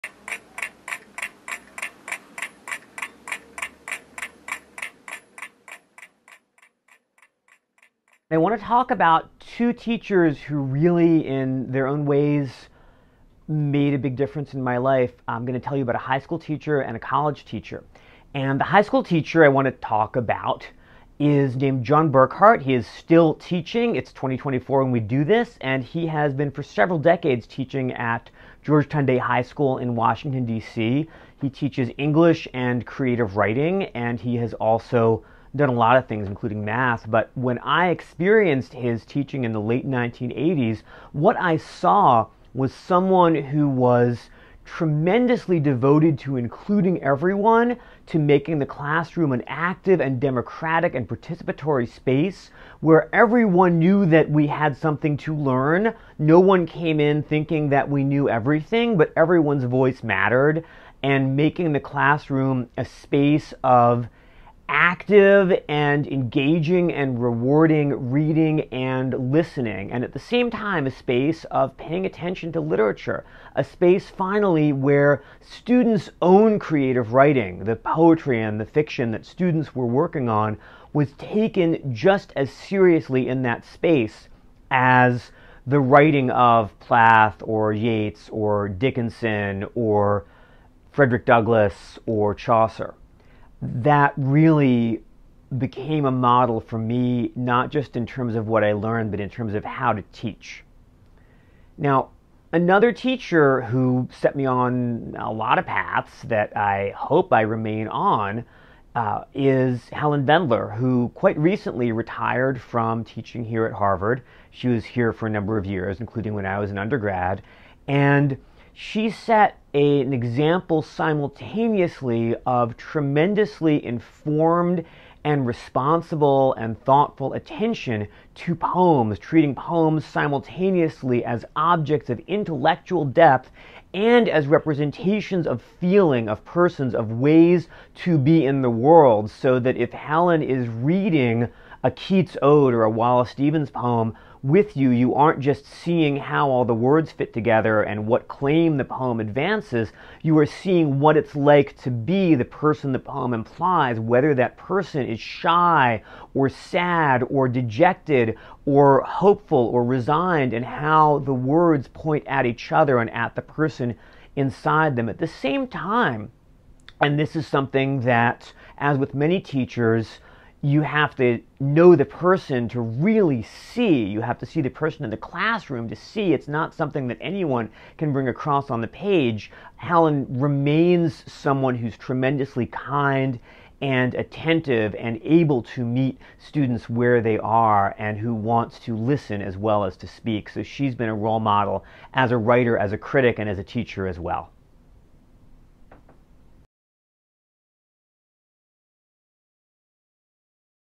Stephanie Burt - Teachers Make a Difference - John Burghardt & Helen Vendler
Harvard Professor Stephanie Burt talks about two teachers who made a difference in her life, one from High School and the other from College.

Stephanie Burt is a literary critic and poet who is Donald P. and Katherine B. Loker Professor of English at Harvard University.The New York Times has called her "one of the most influential poetry critics of [her] generation" Burt grew up around Washington, D.C. She has published various collections of poetry and a large amount of literary criticism and research.[5] Her work has appeared in The New Yorker, The New York Times Book Review, The London Review of Books, and other publications.

Stephanie is currently teaching a course at Harvard called “Taylor Swift and Her World”. Over three hundred students have enrolled.